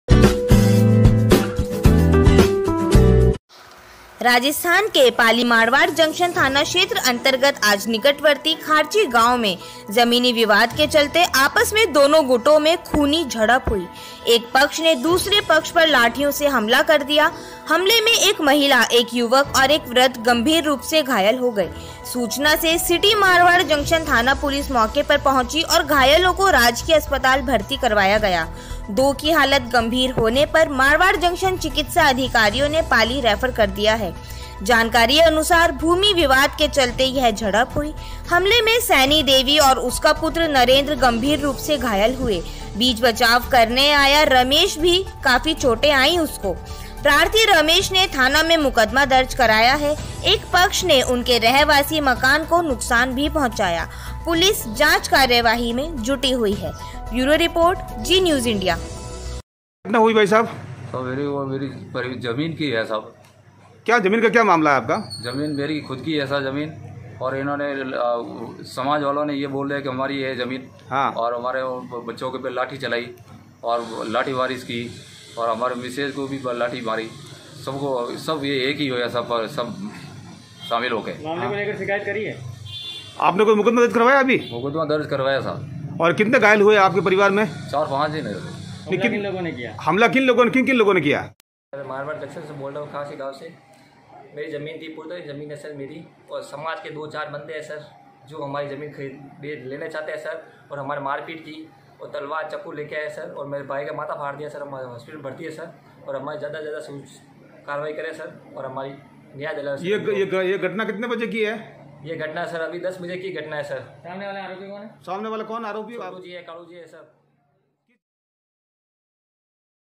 जमीनी विवाद को लेकर दो पक्षों में हुई आपस में खूनी झड़प दो युवक और एक महिला हुए गंभीर रूप से घायल
पाली,,

मारवाड़ जंक्शन थाना क्षेत्र अंतर्गत आज  निकटवर्ती खारची गांव में जमीनी विवाद के चलते आपस में दोनों गुटों में खूनी झड़प हुई एक पक्ष ने दूसरे पक्ष पर लाठियों से हमला कर दिया हमले में एक महिला एक युवक और एक  वृद्ध गंभीर रूप से घायल हो गए ,सूचना से सिटी मारवाड़ जंक्शन थाना पुलिस मौके पर पहुंची और घायलों को राजकीय अस्पताल भर्ती करवाया, दो की हालत गंभीर होने पर मारवाड़ जंक्शन चिकित्सा अधिकारियों ने पाली रेफर कर दिया, जानकारी के अनुसार भूमि विवाद के चलते यह झड़प हुई  हमले में सैनी देवी और उसका पुत्र नरेंद्र गम्भीर घायल हो गया, बीच बचाव करने गया रमेस को भी चोटे आई।
प्राथी रमेस ने  थाना में  मुकदमा दर्ज कराया,एक पक्ष ने उनके  रहवासी मकान को नुकसान भी पहुचाया। पुलिस जांच कार्यवाही में जुटी।

बाईट
1 रमेश प्राथी
2 परिजन तेजाराम